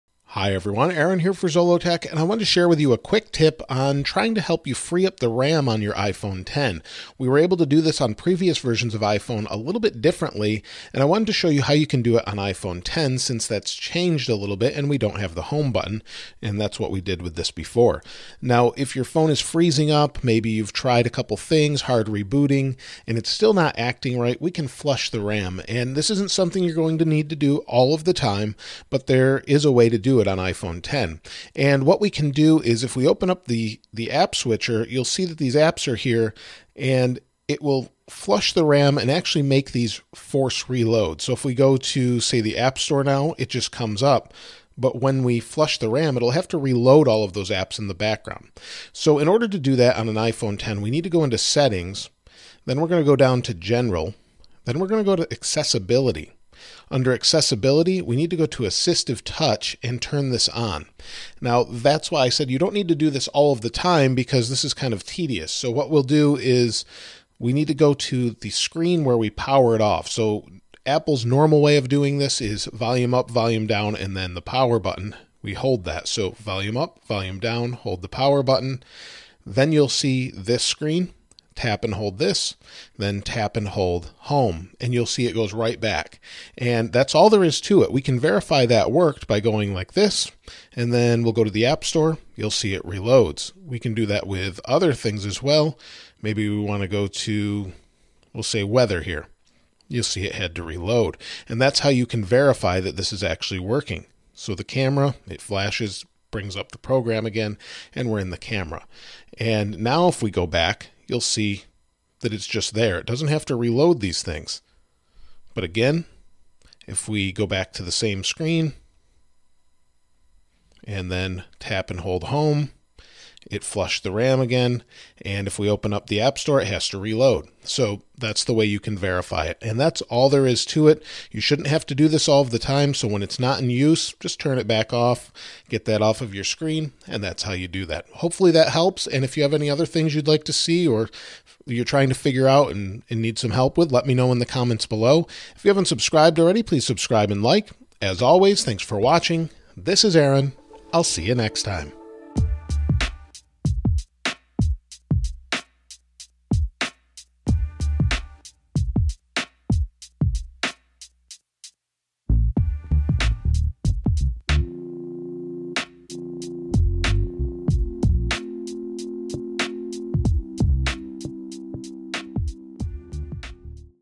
How To Clear iPhone X RAM Memory
I show you how to clear iPhone X RAM to help with closing all apps at once, freezing, lockups and more.